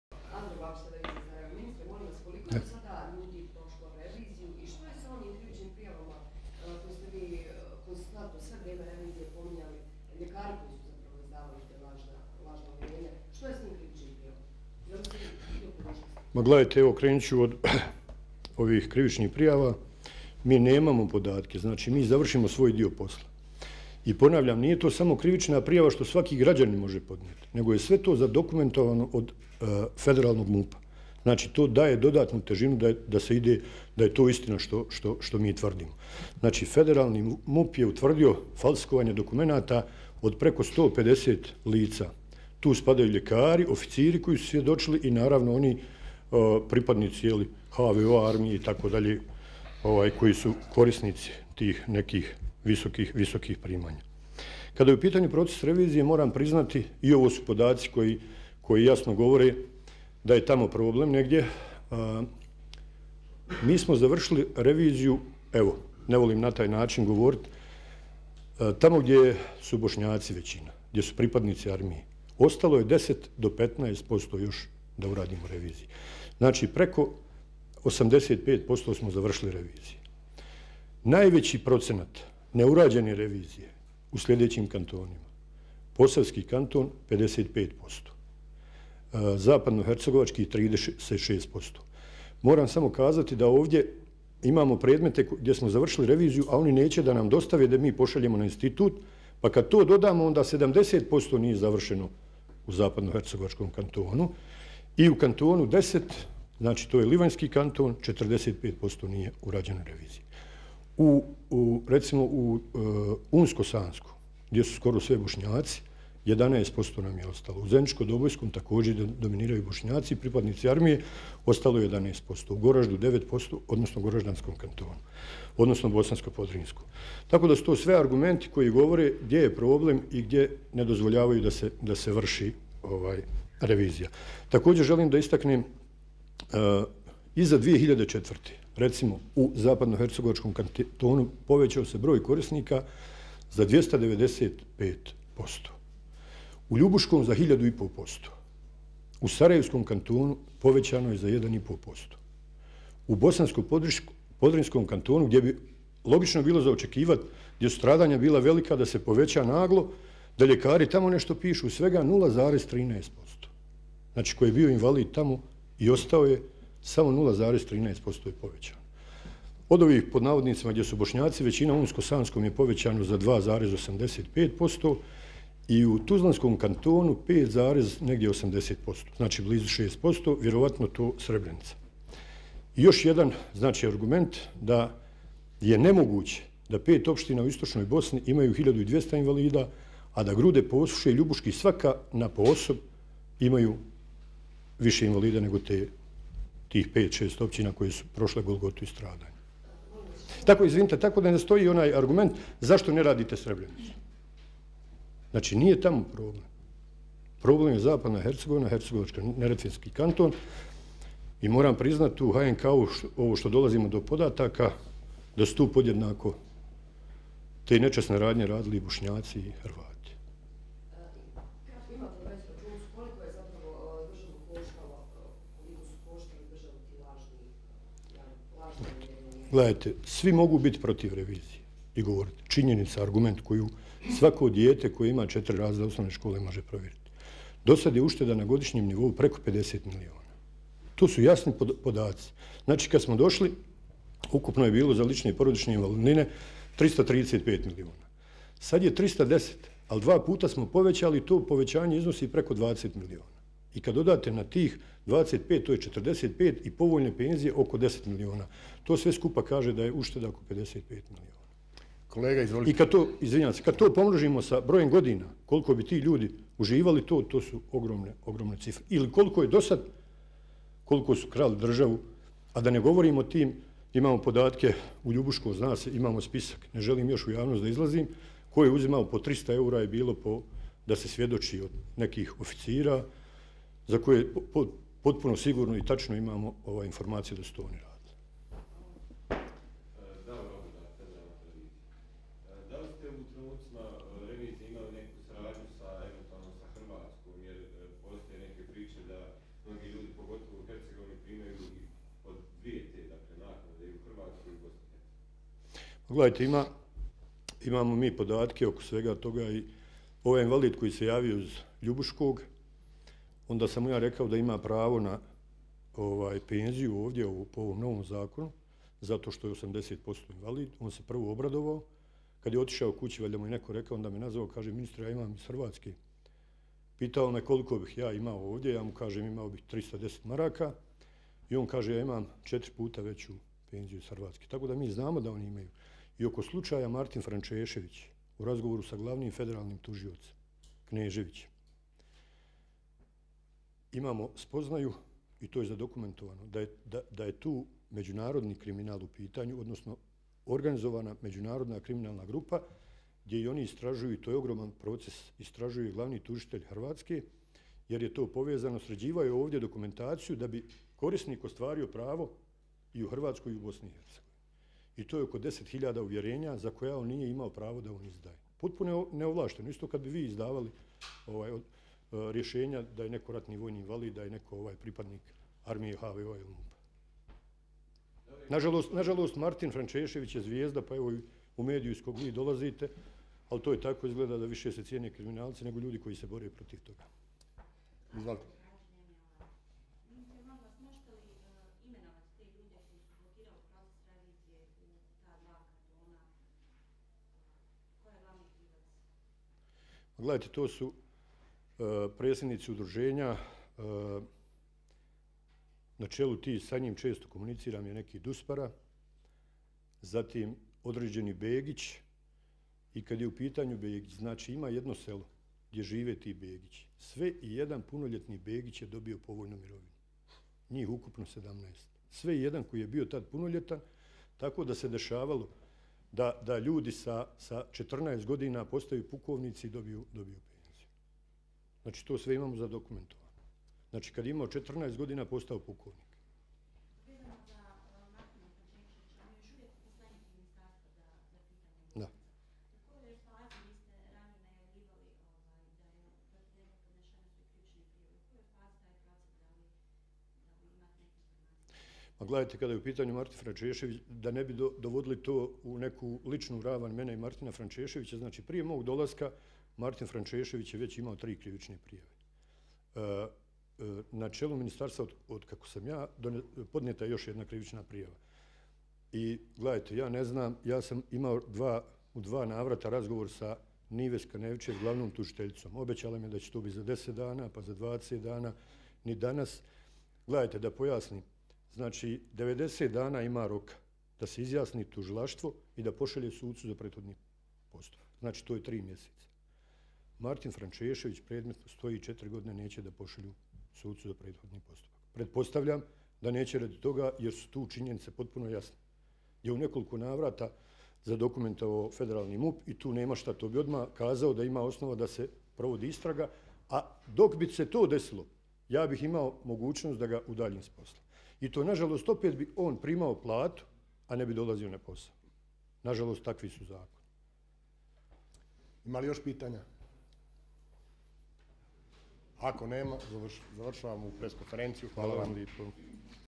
Helez: Federalni MUP je utvrdio falsifikovanje dokumenata preko 150 lica
U falsifikovanju dokumenata ucestvovali ljekari, oficiri i lazni korisnici borackih prava. Provodjenje Zakona o reviziji u svim dijelovima Federacije BiH odvija se nesmetano, osim u opcinama Ljubuski, Posusje, Grude, Orasje i dijelom u Sirokom Brijegu.